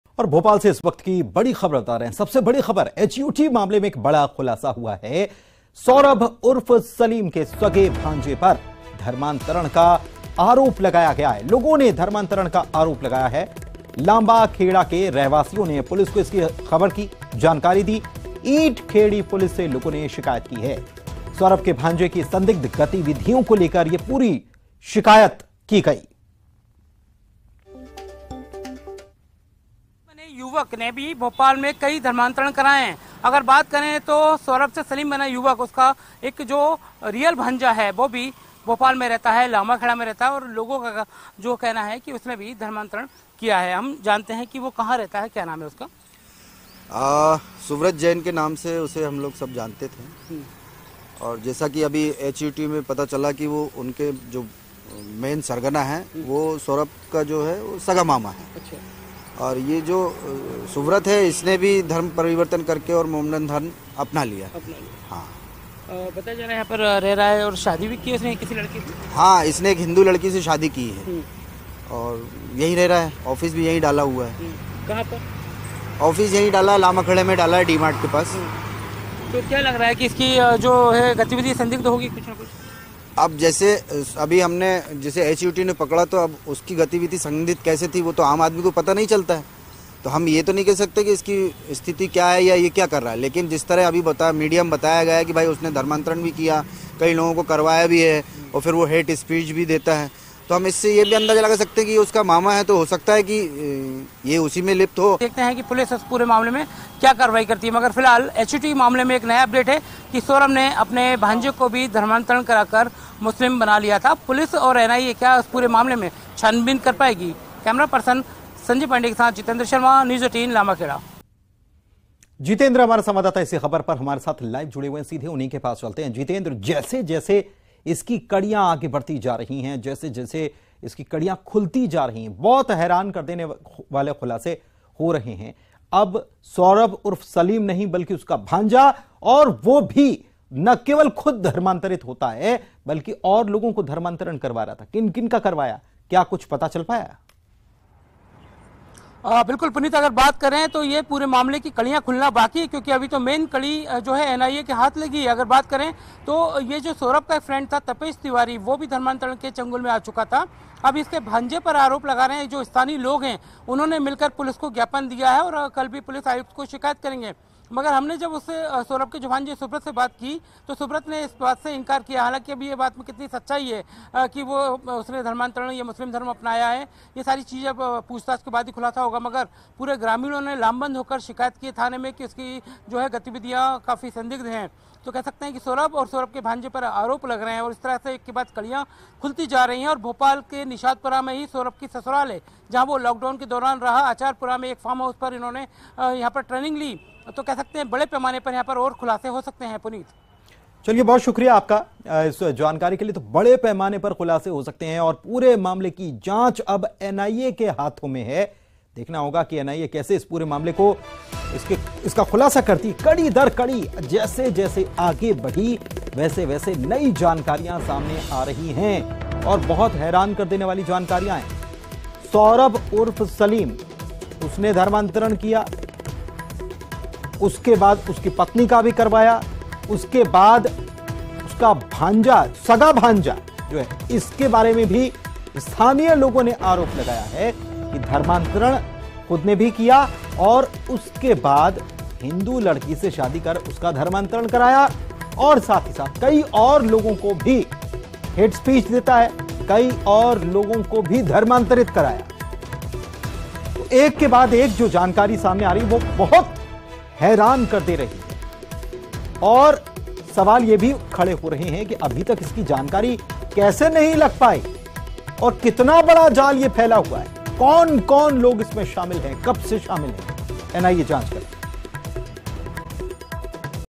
Bhopal News : HUT मामले पर बहुत बड़ा खुलासा | Zakir Naik | Saurabh Alias Salim | Conversion
Bhopal News : HUT मामले पर बहुत बड़ा खुलासा, सौरभ उर्फ सलीम के सगे भांजे पर धर्मांतरण का आरोप. लोगों ने लगाया धर्मांतरण का आरोप. देखिए क्या है पूरा मामला.